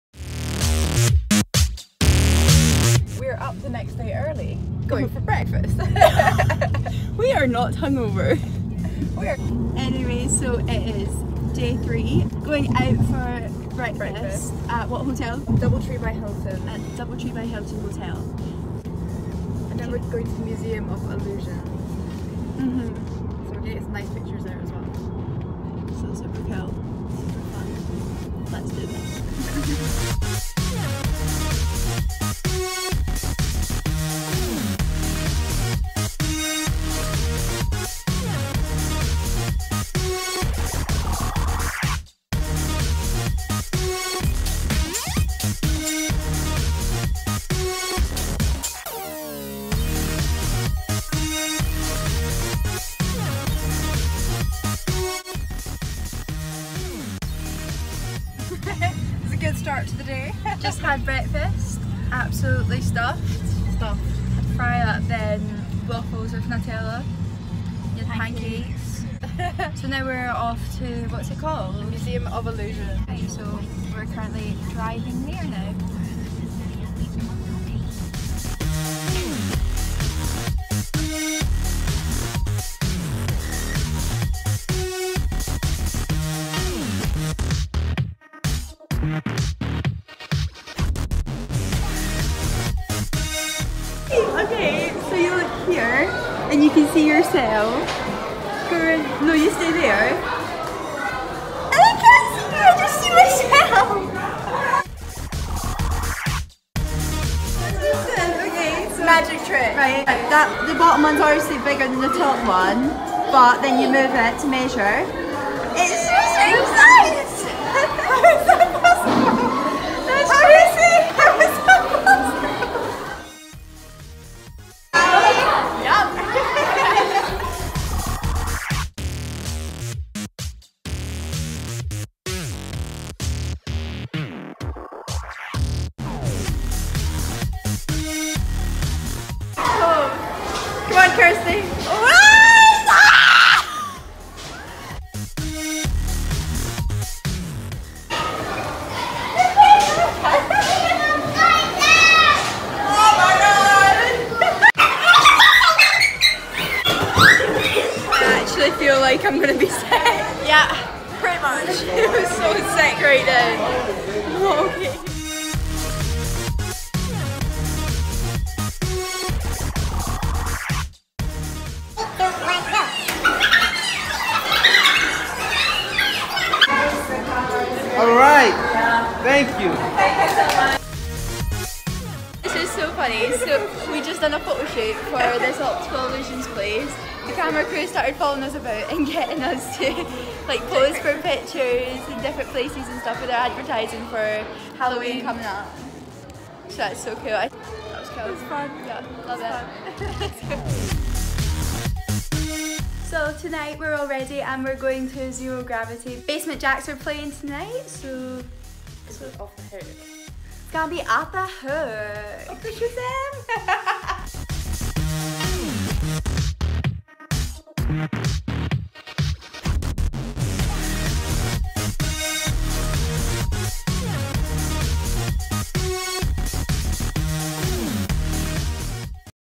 Illusions& Basement Jaxx@Zero Gravity- DUBAI VLOG 3!!
I’m currently on back holiday visiting my sister in Dubai for 12 days so going to be doing a vlogging series of what we get up to every day on our holiday!!! Certainly be ready to see some exciting, hilarious, beautiful and interesting events that take place on my holiday!!

This is day 3 19/10/2018- Today we went to breakfast at Double Tree by Hilton and had a lovely brunch breakfast. We then went down old Dubai to The Creek and went to the museum of illusions!! That place was insane! And then at night we went to dinner with friends and went to Zero Gravity beach club to see Basement Jaxx dj, was an amazing day!!

X